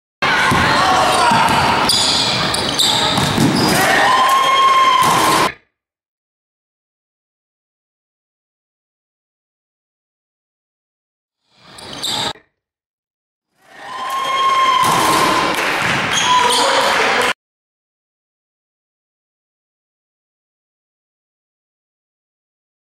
Freshman Houston Smith Connects On Alley Oop From Trevell Beck
All Ohio Red 15s.